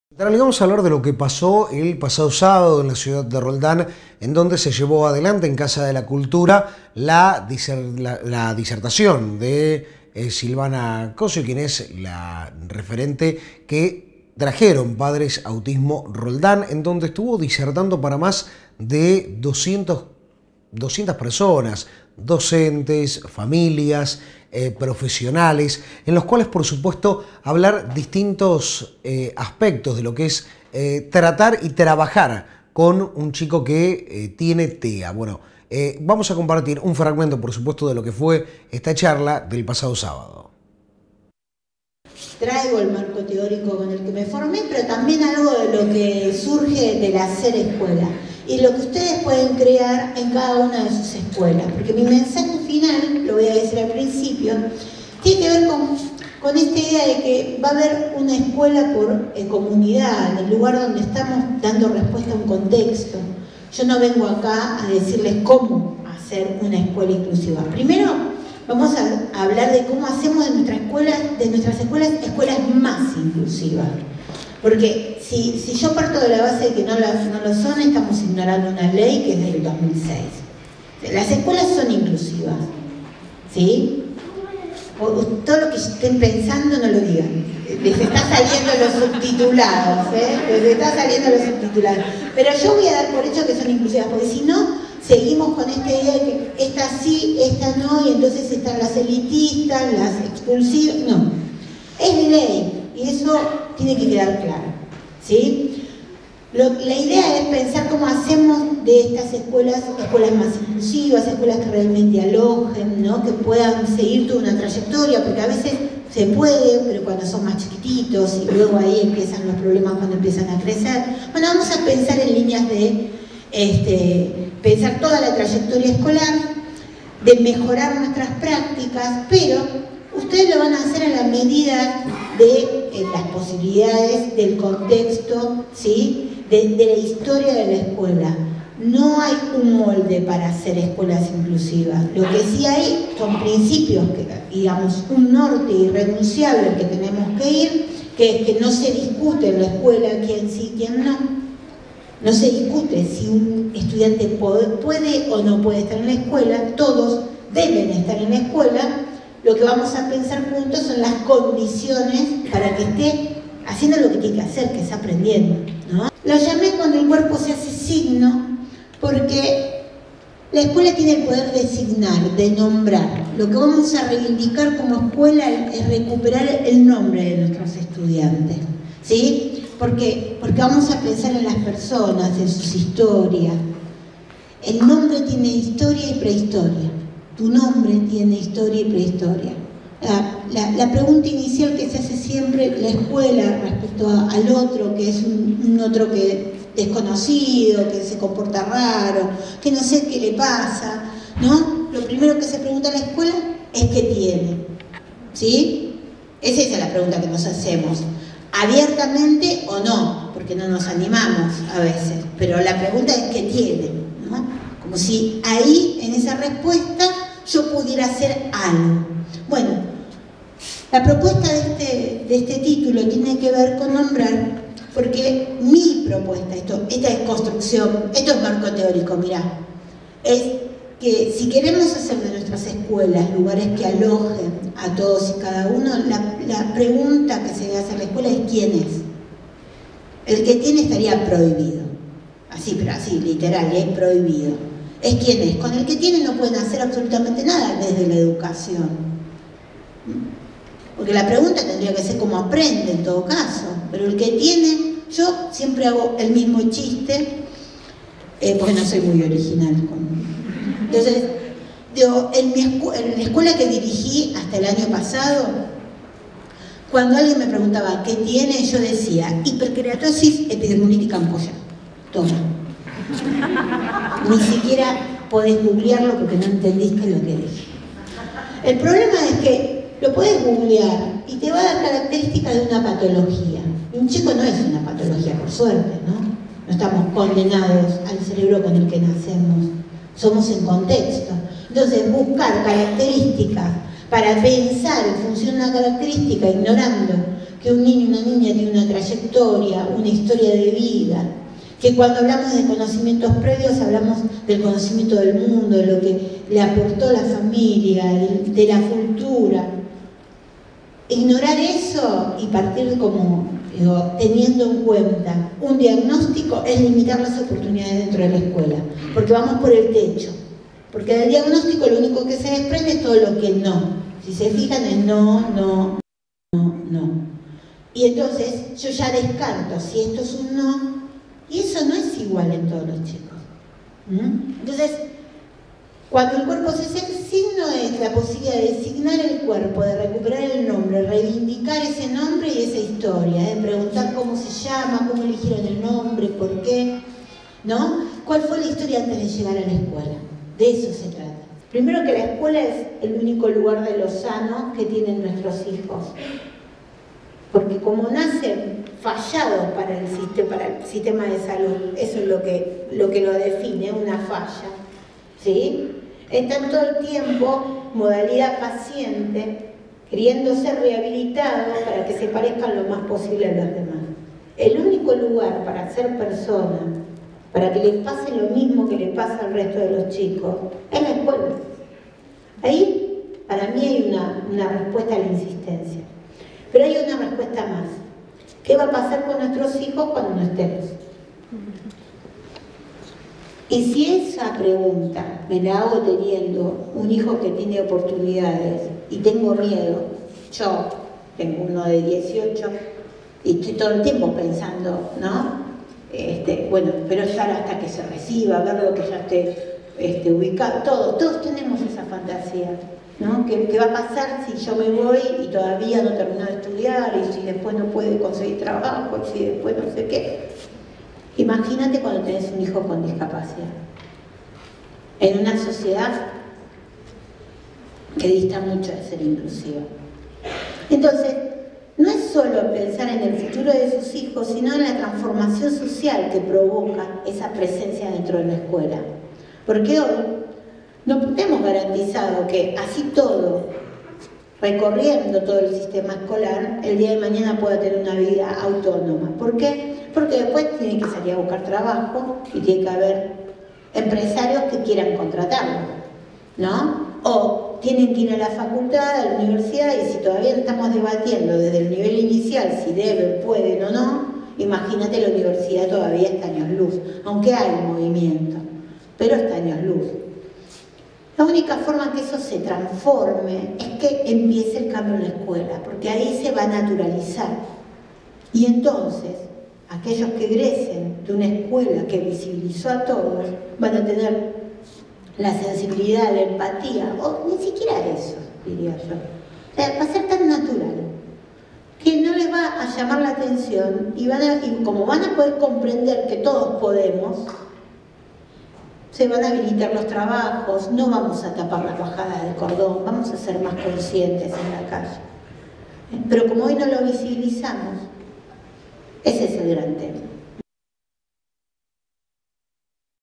Conferencia de Silvana Corso sobre Educación Inclusiva
Organizada por Padres Autismo Roldán, la reconocida educadora Silvana Corso brindó una disertación acerca de la Educación Inclusiva.